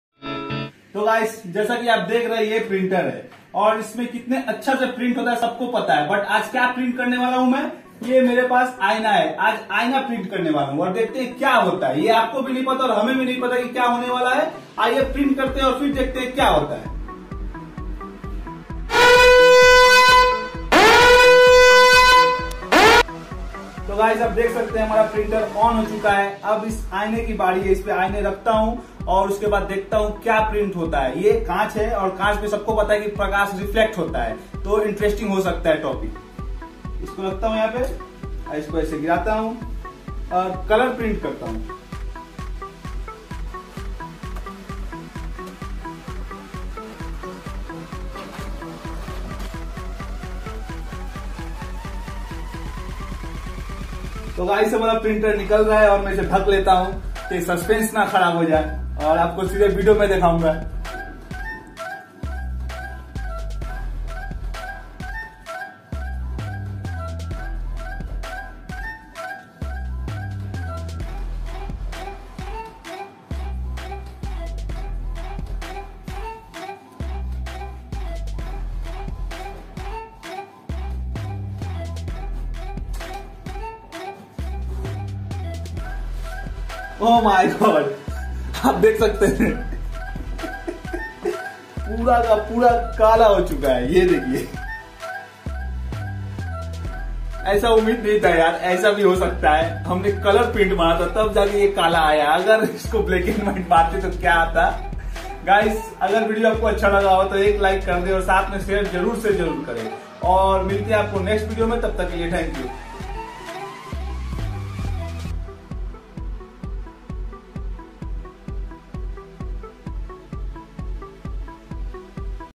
मैने आईने को प्रिन्ट किया | Mirror Printing Experiment
Hello Dosto, Aaj ek baar phir apke liye ek jabardast experiment taiyar hai.